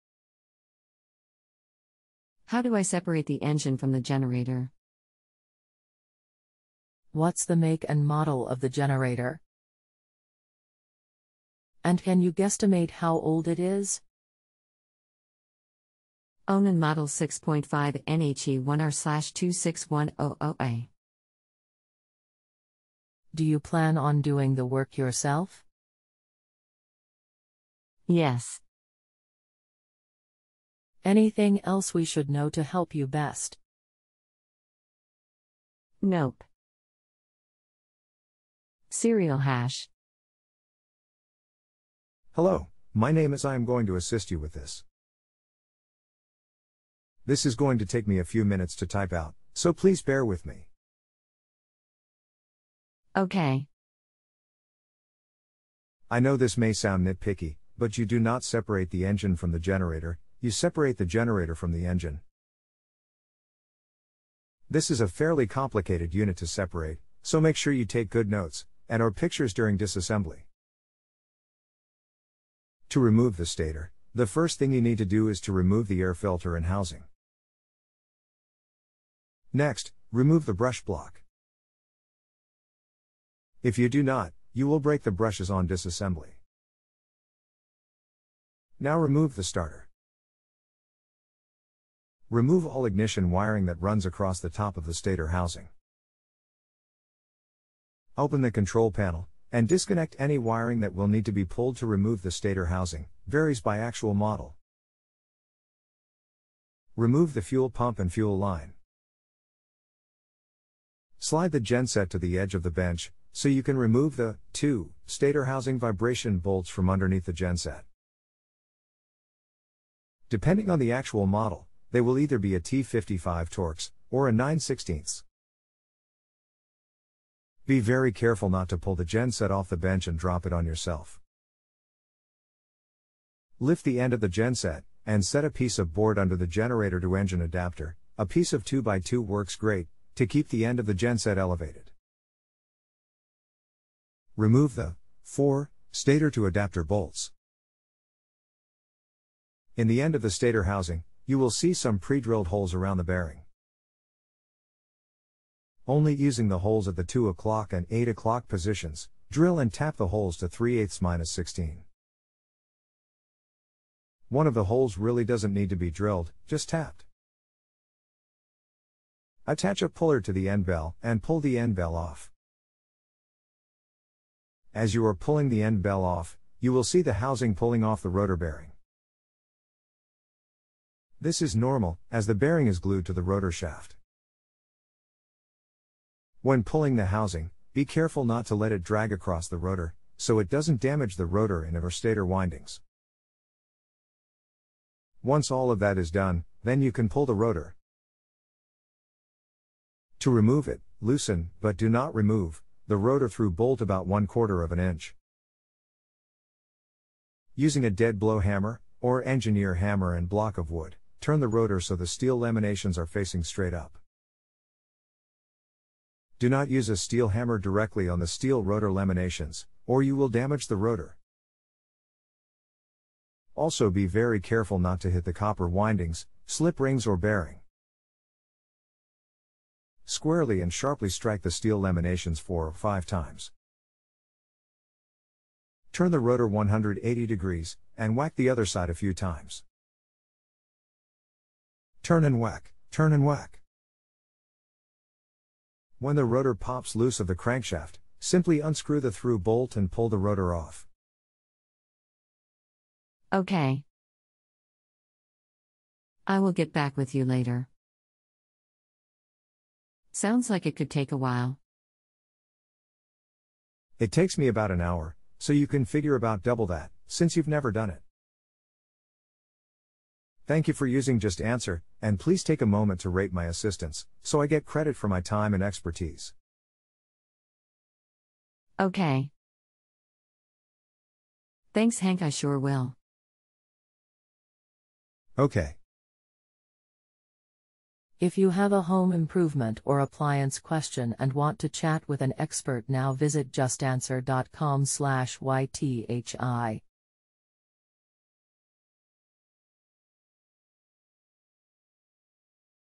How do I seperate the engine from the generator? Onan Model
JustAnswer Customer: How do I seperate the engine from the generator?
Pearl Wilson: Assistant: What's the make and model of the generator?
Pearl Wilson: Assistant: And can you guesstimate how old it is?
JustAnswer Customer: Onan Model 6.5 NHE-1R/26100A
JustAnswer Customer: Yes
JustAnswer Customer: Nope
JustAnswer Customer: Serial# *
Hank F.: Technician: Hello, my name is * * I am going to assist you with this.
Hank F.: Technician: This is going to take me a few minutes to type out, so please bear with me.
JustAnswer Customer: Ok
Hank F.: Technician: I know this may sound nit-picky, but you do not separate the engine from the generator - you separate the generator from the engine.
Hank F.: Technician: This is a fairly complicated unit to separate, so make sure you take good notes, and/or pictures during disassembly.
Hank F.: Technician: To remove the stator, the first thing you need to do is to remove the air filter and housing.
Hank F.: Technician: Next, remove the brush block.
Hank F.: Technician: If you do not, you will break the brushes on disassembly.
Hank F.: Technician: Now remove the starter.
Hank F.: Technician: Remove all ignition wiring that runs across the top of the stator housing.
Hank F.: Technician: Open the control panel, and disconnect any wiring that will need to be pulled to remove the stator housing (varies by actual model).
Hank F.: Technician: Remove the fuel pump and fuel line.
Hank F.: Technician: Slide the genset to the edge of the bench, so you can remove the (2) stator housing vibration bolts from underneath the genset.
Hank F.: Technician: Depending on the actual model, they will either be a T-55 Torx, or a 9/16”.
Hank F.: Technician: Be very careful not to pull the genset off the bench and drop it on yourself.
Hank F.: Technician: Lift the end of the genset, and set a piece of board under the generator to engine adapter (a piece of 2x2 works great), to keep the end of the genset elevated.
Hank F.: Technician: Remove the (4) stator to adapter bolts.
Hank F.: Technician: In the end of the stator housing, you will see some predrilled holes around the bearing.
Hank F.: Technician: ONLY using the holes at the 2 o’clock and 8 o’clock positions, drill and tap the holes to 3/8”-16.
Hank F.: Technician: One of the holes really doesn’t need to be drilled, just tapped.
Hank F.: Technician: Attach a puller to the end bell, and pull the end bell off.
Hank F.: Technician: As you are pulling the end bell off, you will see the housing pulling off the rotor bearing.
Hank F.: Technician: This is normal, as the bearing is glued to the rotor shaft.
Hank F.: Technician: When pulling the housing, be careful not to let it drag across the rotor, so it doesn’t damage the rotor and/or stator windings.
Hank F.: Technician: Once all of that is done, then you can pull the rotor.
Hank F.: Technician: To remove it, loosen, but do not remove, the rotor through bolt about 1/4".
Hank F.: Technician: Using a dead blow hammer, or engineer hammer and block of wood, turn the rotor so the steel laminations are facing straight up.
Hank F.: Technician: DO NOT USE A STEEL HAMMER DIRECTLY ON THE STEEL ROTOR LAMINATIONS, OR YOU WILL DAMAGE THE ROTOR.
Hank F.: Technician: Also be very careful not to hit the copper windings, slip rings or bearing.
Hank F.: Technician: Squarely and sharply strike the steel laminations 4 or 5 times.
Hank F.: Technician: Turn the rotor 180 degrees, and whack the other side a few times.
Hank F.: Technician: Turn and whack, turn and whack.
Hank F.: Technician: When the rotor pops loose of the crankshaft, simply unscrew the through bolt and pull the rotor off.
JustAnswer Customer: Ok.
JustAnswer Customer: I will get back with you later!
JustAnswer Customer: Sounds like it could take awhile!
Hank F.: Technician: It takes me about an hour, so you can figure about double that, since you've never done it.
Hank F.: Technician: Thank you for using Just Answer, and please take a moment to rate my assistance, so I get credit for my time and expertise.
JustAnswer Customer: OK.
JustAnswer Customer: Thanks Hank I sure will!
Hank F.: Technician: Ok.